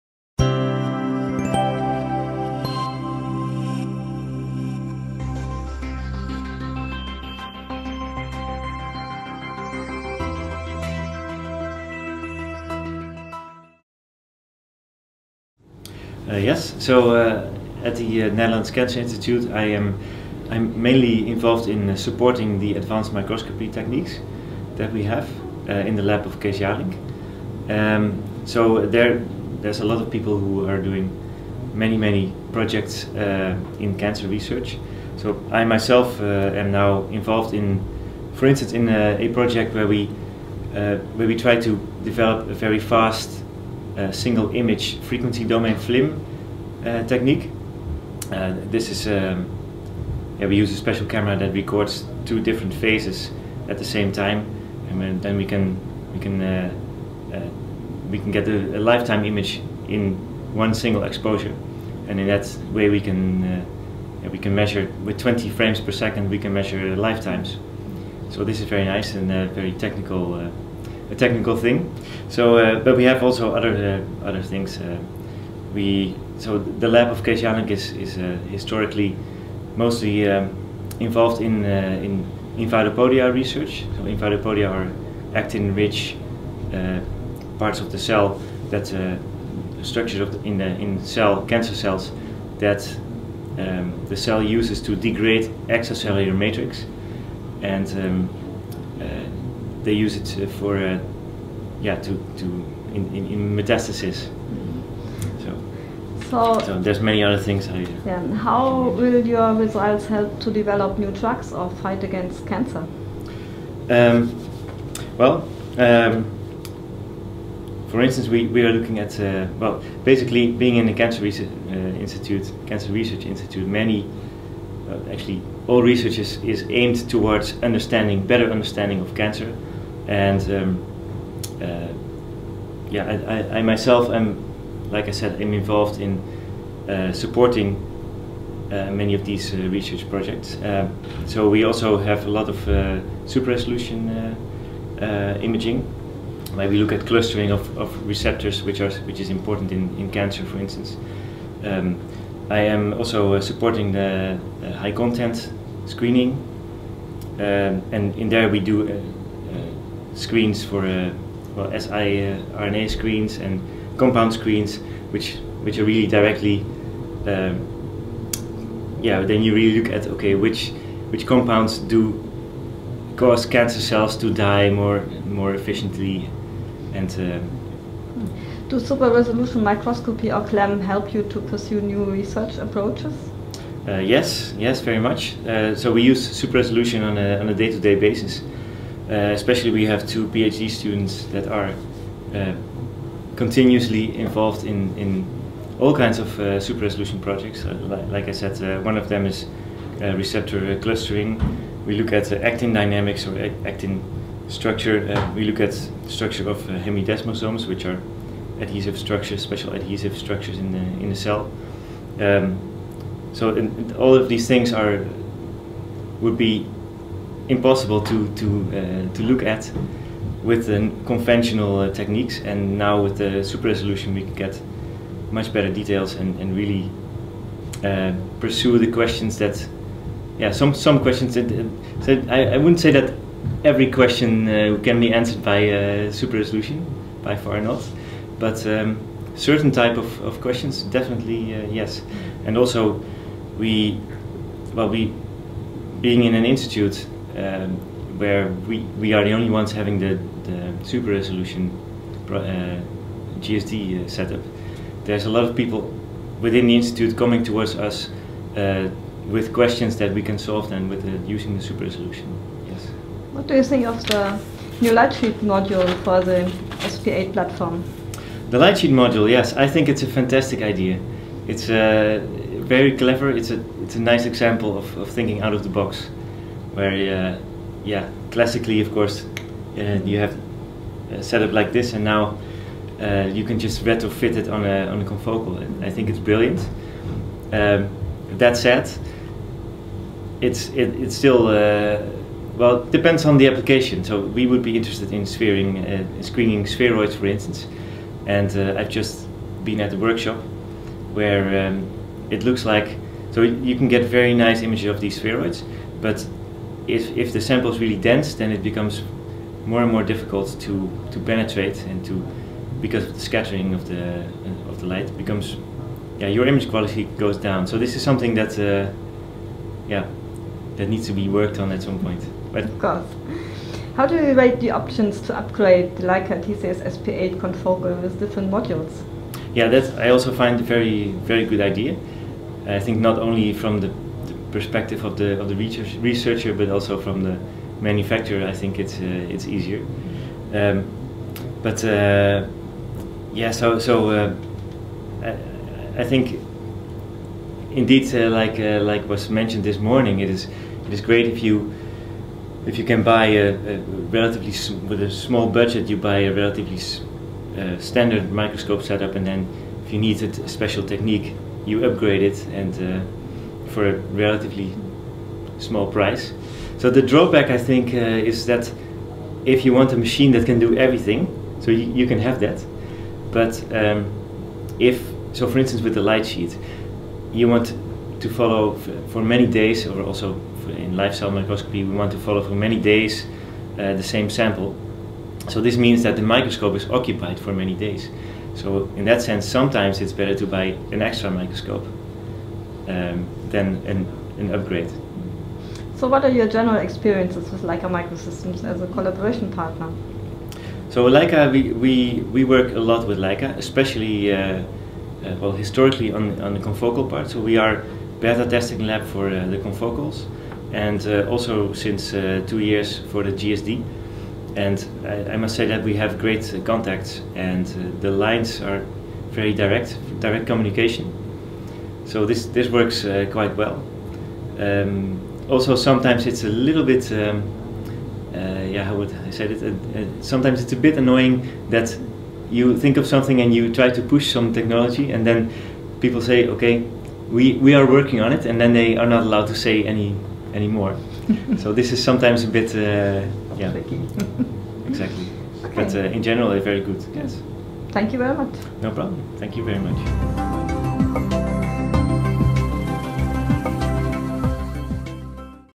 "The Leica TCS SP8 DLS - a Clever Example of Thinking Out of the Box"
Leica Microsystems meets Science: Interview with Bram van den Broek, Netherlands Cancer Institute (NKI), Amsterdam, The Netherlands.

Bram van den Broek is a postdoctoral fellow at the Netherlands cancer institute in Amsterdam where he supports the advanced microscopy techniques in the laboratory of Kees Jalink. His main project is developing a fast single image frequency domain FLIM technique. But he is also involved I many other research projects, such as super-resolution imaging of receptor clustering to better understand cancer or high content microscopy screens in search for siRNAs or compounds that induce cell death in cancer cells. He likes the platform principle of the Leica TCS SP8 as it allows to develop the microscopy with upcoming demands. In his opinion, the Leica TCS SP8 DLS light sheet module, which can be retrofitted to an inverted confocal microscope, is a clever example of thinking out of the box. Working with Leica Microsystems as a collaboration partner for beta-testing of microscopes he enjoys very much and praised the open and direct communication.

For more information on microscope solutions from Leica Microsystems, please go